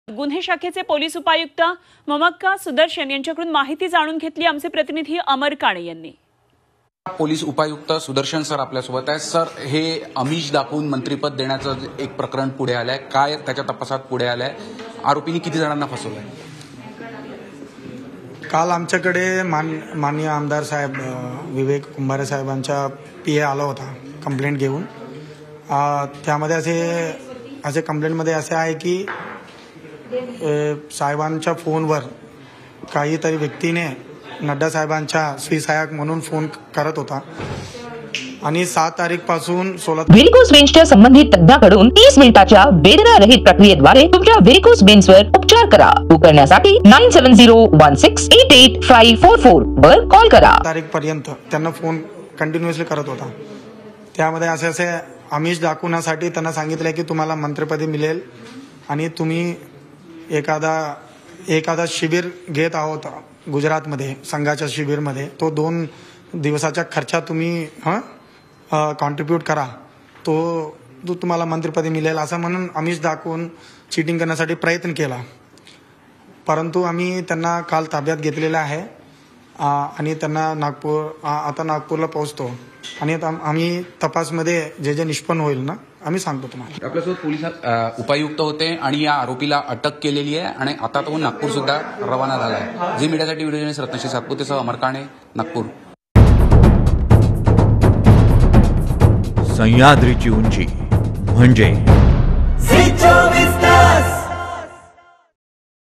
BJP MLAs get Fake Call | मंत्रीपदाचे अमिष दाखवून कोण लुटतंय आमदारांना? पाहा पोलिस काय म्हणाले
मंत्रिपदाचे आमिष दाखवून आमदारांची फसवणूक केल्याचा धक्कादायक प्रकार समोर आलाय...जे.पी.नड्डांच्या जवळचा असल्याचं सांगून 6 आमदारांना फोनवरून संपर्क करण्यात आला...आणि पैशांची मागणी करण्यात आली...4 आमदार महाराष्ट्रातील आहेत तर इतर दोन गोवा आणि नागालँडमधील आहेत...नीरजसिंह राठोड असं फसवणूक करणा-याचं नाव आहे...याला गुजरातच्या मोरबीतून ताब्यात घेण्यात आलंय...या आरोपीने भाजपचे बदनापूरचे आमदार नारायण कुचे यांना फोन करून पैसे उकळलेयत...तर आमदार विकास कुंभारे, टेकचंद, सावरकर, तानाजी मुटकुळे यांना फोन करून पैशांची मागणी केली...

A shocking case of cheating the MLAs with the lure of ministership has come to light...6 MLAs were contacted by phone claiming to be close to J.P. Nadda...and demanded money...4 MLAs are from Maharashtra and the other two are from Goa and Nagaland. ..The fraudster's name is Neerajsingh Rathod...He has been arrested from Morbi, Gujarat...The accused called BJP Badnapur MLA Narayan Kuche and extorted money...MLAs Vikas Kumbhare, Tekchand, Savarkar, Tanaji Mutkule. Called and demanded money...

Social Media Links :
5 Minitat 25 Batmya । 5 मिनिटांत 25 बातम्या